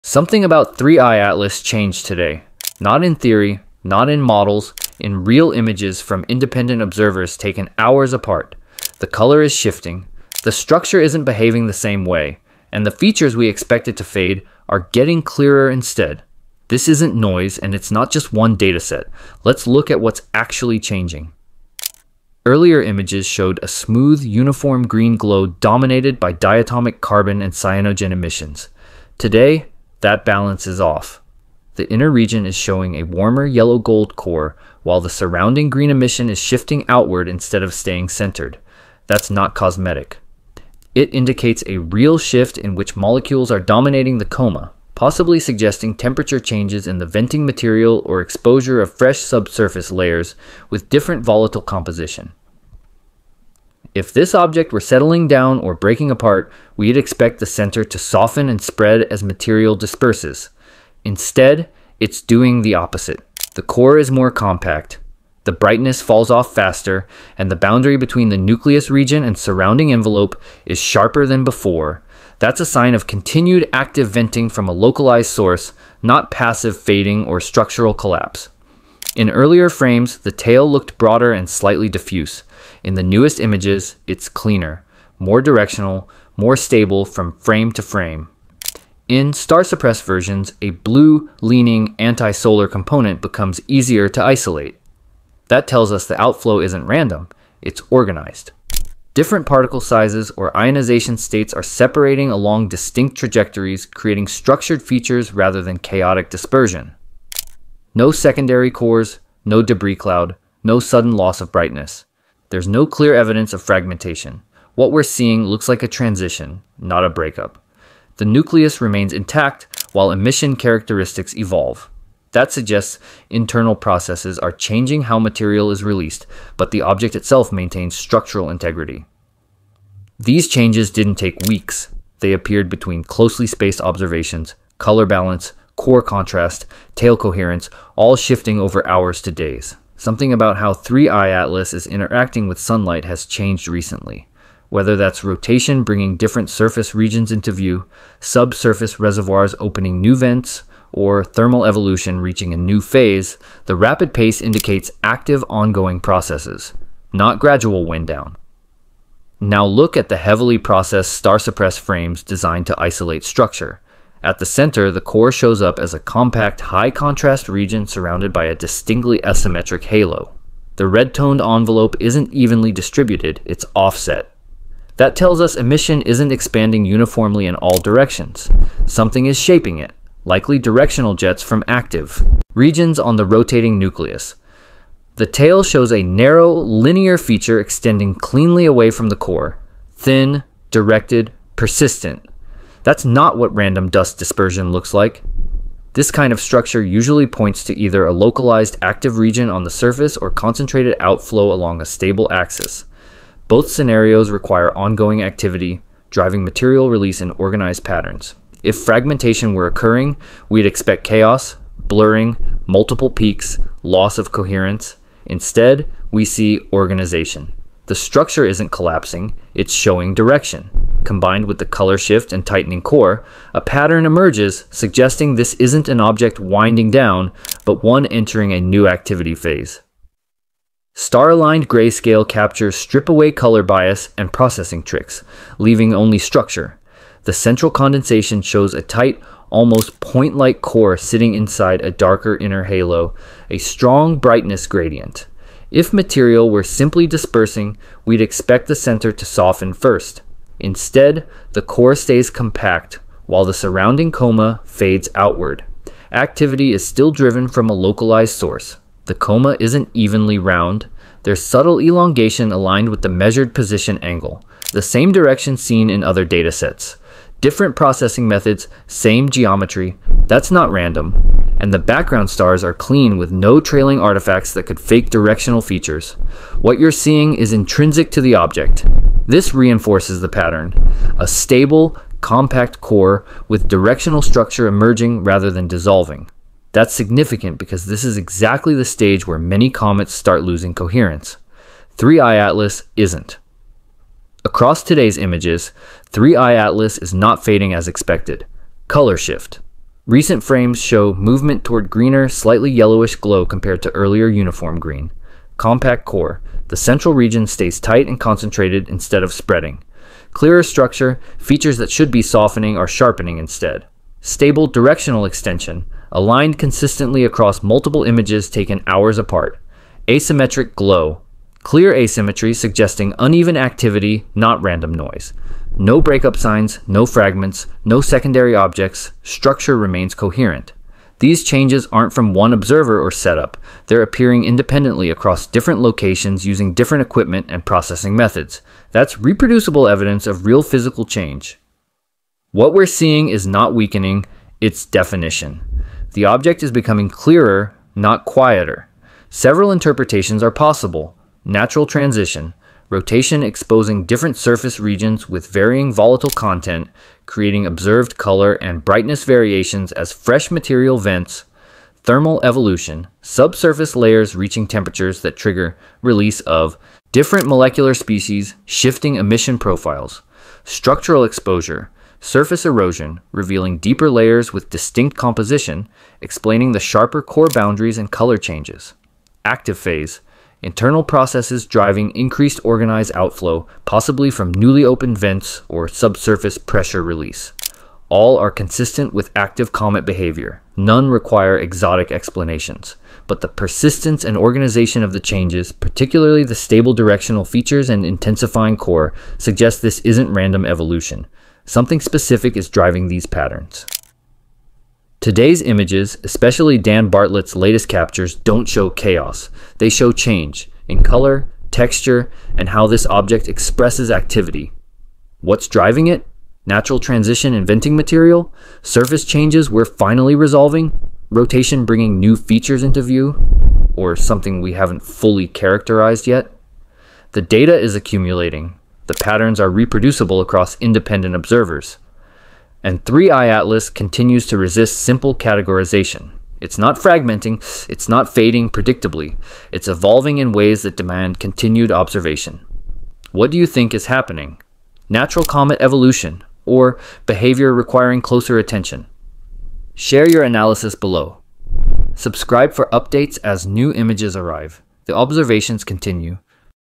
3I/ATLAS Update: New Images REVEAL Color Shift & Compact Core — Unexpected Changes Detected
3I/ATLAS shows rapid changes in new images—shifting from green to yellow-gold, forming a tighter core and clearer, directional structures. Independent observations confirm active venting, organized outflow, and no fragmentation. This evolving interstellar object is entering a new activity phase.